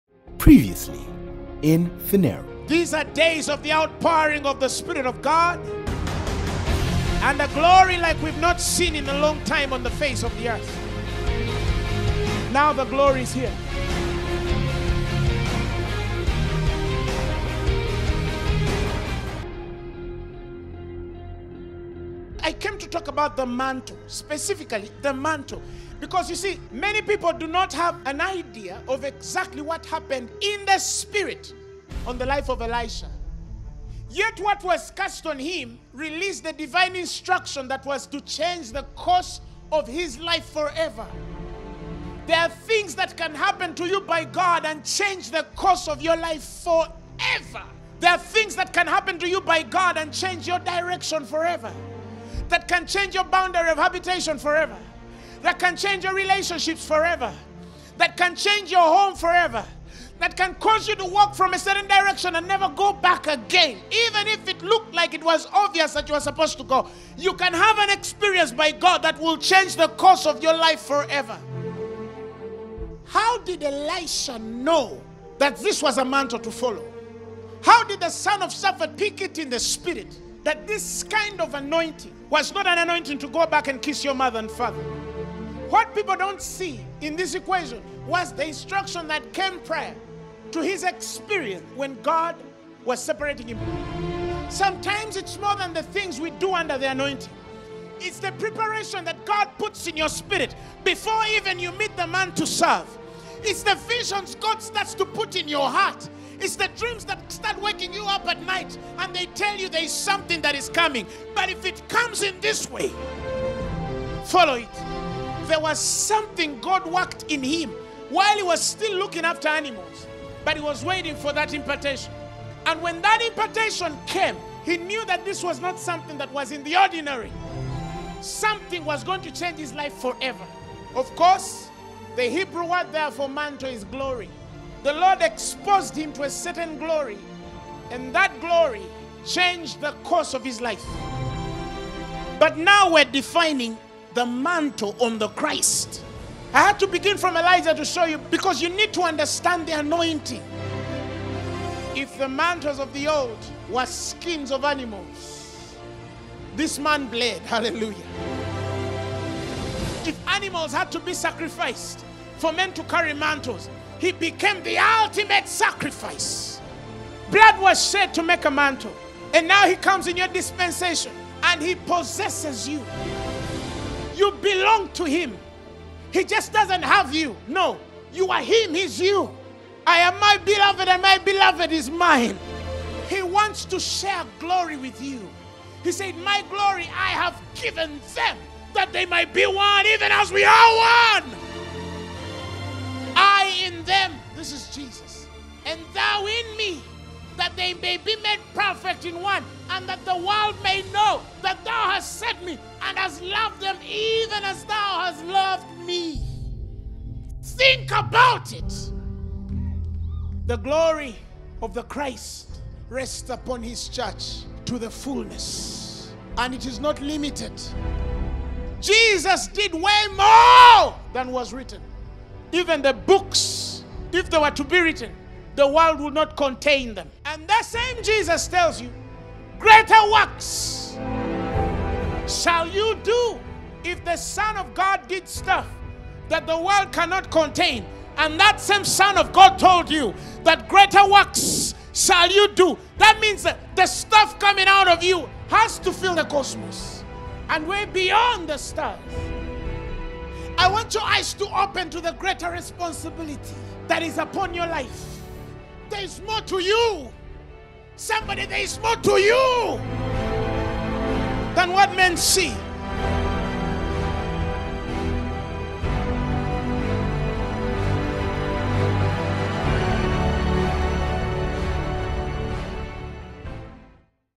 The Mantle Of Christ | Sermon Preview | Apostle Grace Lubega
The Mantle Of Christ | Phaneroo Service 206 Sermon Preview by Apostle Grace Lubega

Watch Apostle Grace Lubega as He talks about the things that can happen to you and change the course of your life forever.

We dive deep into the Mantle of Elijah passed down to Elisha as we understand the Mantle and Anointing upon the Christ, being the ultimate sacrifice.